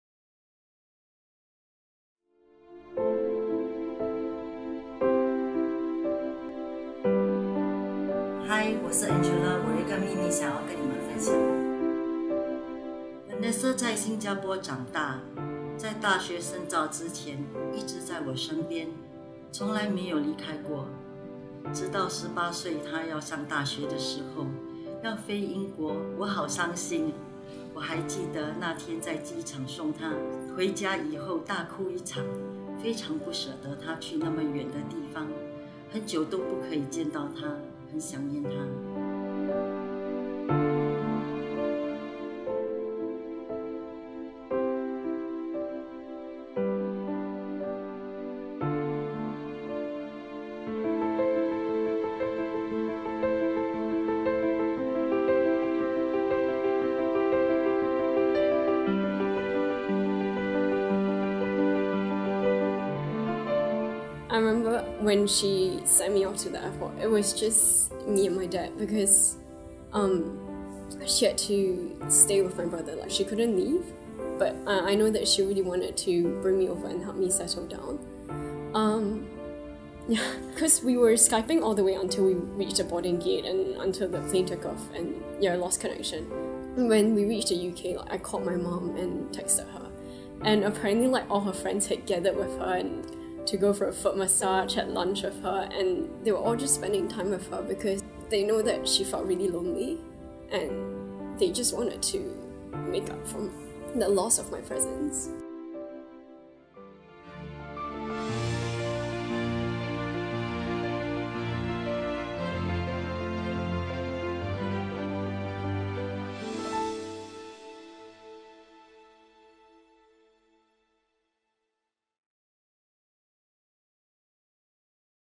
Watch reactions when best-kept secrets are revealed between mother-daughter Angela Ng × Vanessa Ng
本地名媛李黛萍(Angela Ng)的女儿黄曦慜(Vanessa Ng)今年22岁，刚从英国埃克塞特大学(University of Exeter)念完法学学士的她回国发展，作为新晋律师进入职场三个多月。从小无话不说的母女向《ICON风华》分享分别与陪伴的泪水与幸福。